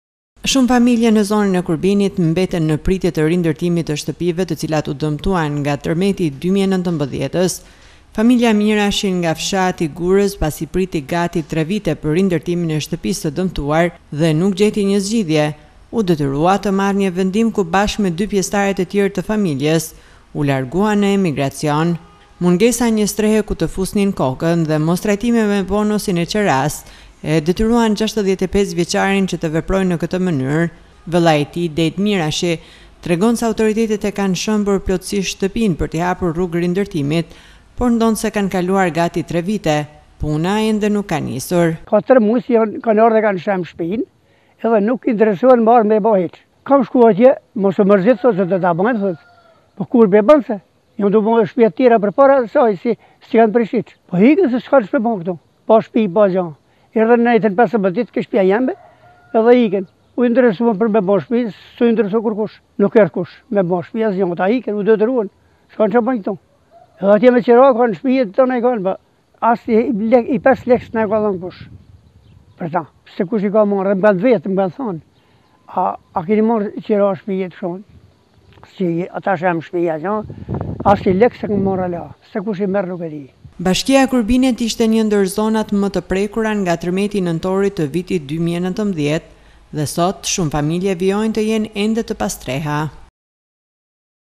TERMETI I NENTORIT 2019, 3 VITE PA NJE ÇATI MBI KOKE
Shumë familje në zonën e Kurbinit janë në ende në pritje të rindërtimit të shtëpive të cilat u dëmtuan nga tërmeti. Familja Mirashi nga fshati Gurëz pasi priti gati 3 vite për rindërtimin e shtëpisë së dëmtuar nga tërmeti dhe nuk gjeti zgjidhje, u detyrua të marrë një vendim ku bashkë me 2 pjestarët e tjerë të familjes u largua në emigracion.